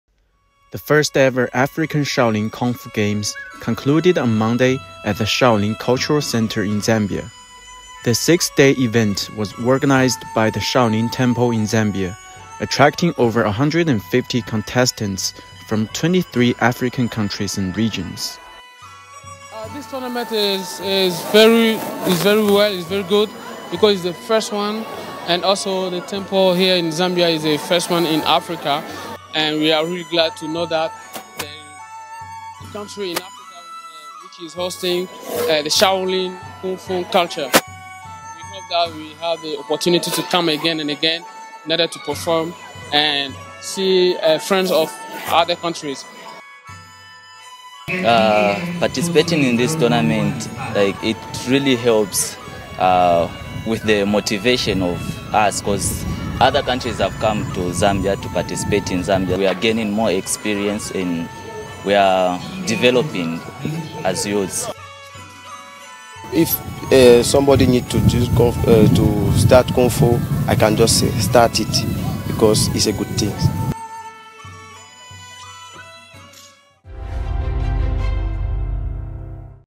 GLOBALink | 1st African Shaolin Kung Fu Games concludes in Zambia
The first-ever African Shaolin Kung Fu Games concluded on Monday in Zambia, attracting over 150 contestants from 23 African countries and regions.